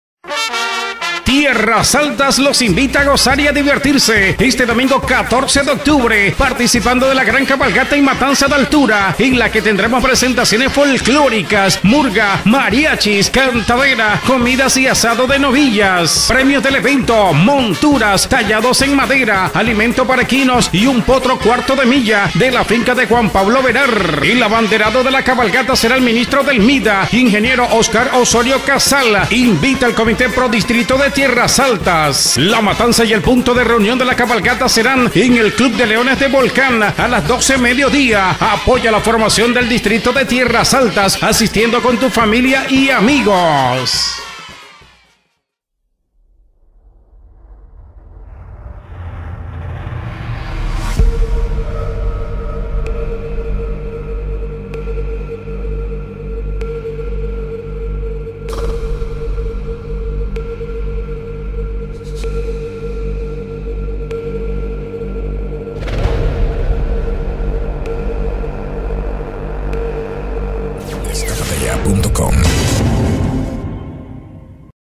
CABALGATA Y MATANZA 14 DE OCTUBRE SKAPATEYA.COM
Tierras Altas los invita a gozar y divertirse el Domingo 14 de Octubre. Participando de la gran cabalgata y matanza de altura, en la que tendremos presentaciones folklóricas, murga, mariachis, cantadera, comidas y asado de novillas. Premios del evento  monturas, tallados en madera, alimento para equinos y un potro cuarto de milla de la finca de Juan Pablo Berard. El abanderado de la cabalgata será el Ministro del MIDA, Ing. Oscar Osorio Casal. Invita el comité Pro Distrito de Tierras Altas.

La matanza y el punto de reunión de la cabalgata serán en el Club de Leones de Volcán a las 12:00 MD. Apoya la formación del Distrito de Tierras Altas, asistiendo con tu familia y amigos.